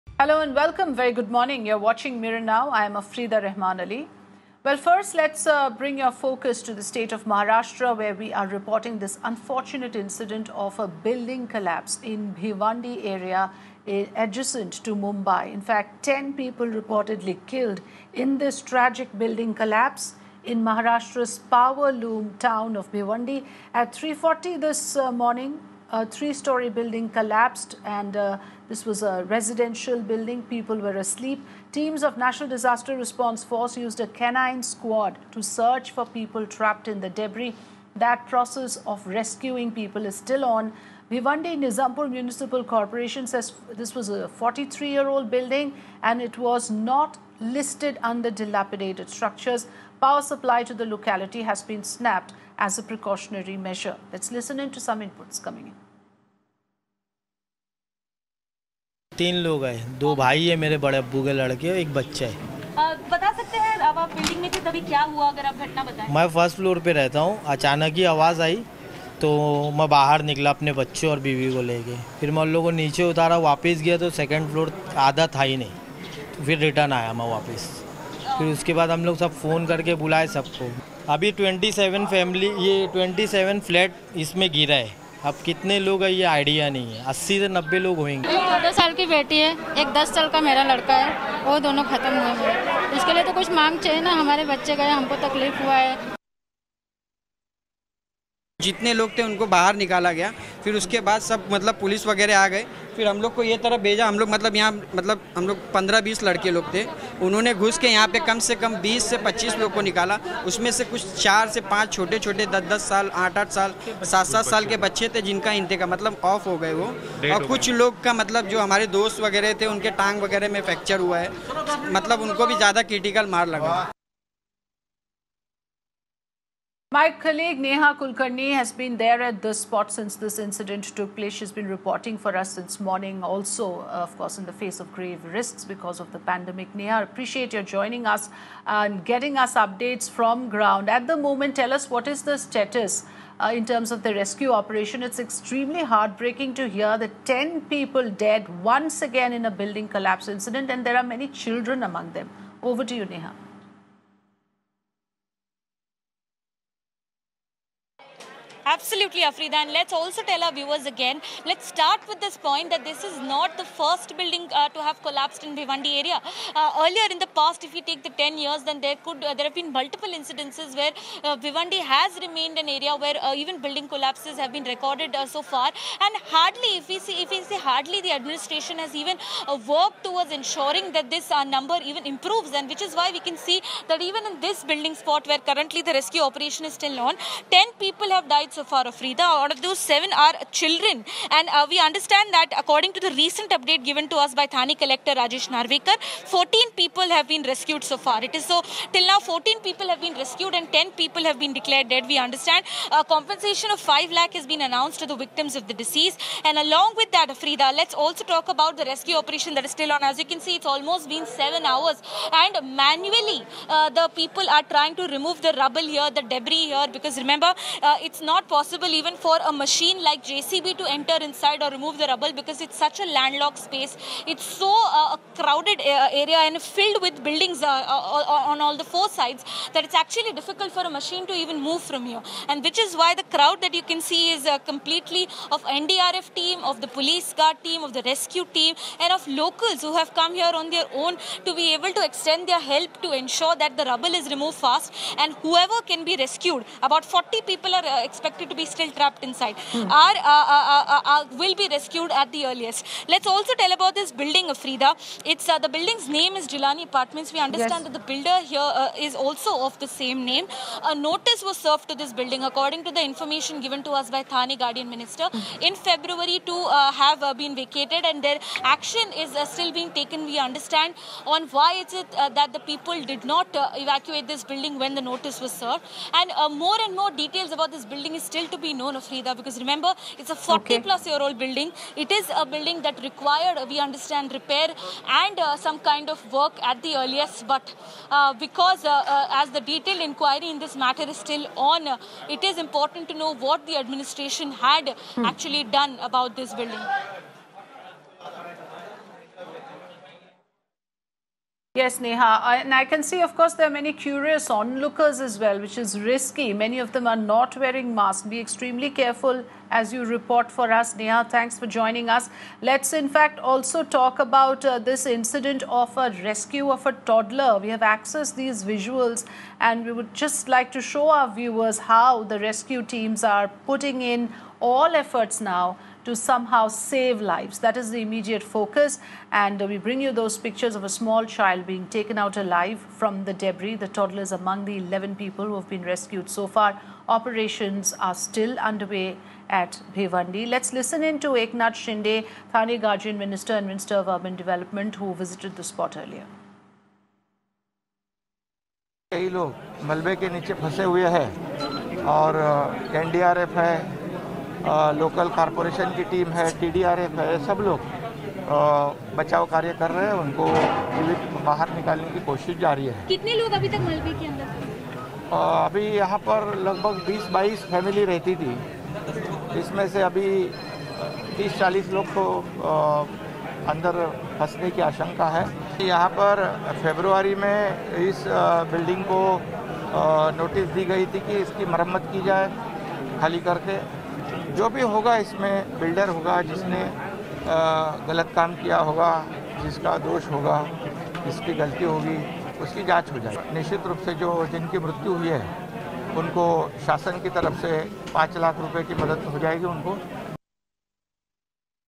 Bhiwandi: 3 storey building collapses when residents were asleep; 11 rescued & several trapped
An unfortunate incident of building collapse in Bhiwandi area adjacent to Mumbai. Ten people reportedly killed in this tragic building collapse in Maharashtra's power loom town in Maharashtra. At 3:40 AM 3 storey building collapsed and it was a residential building and people were asleep.

Teams of disaster management came in for the rescue and searched for the people who are trapped in the debris and the process of rescuing people is still on. Bhiwandi Nizampur City Municipal Corporation said this building for 43 years old and was listed under the dilapidated structures. Power supply to the locality has been snapped as a precautionary measure.

Mirror Now's speaks to the residents of the building.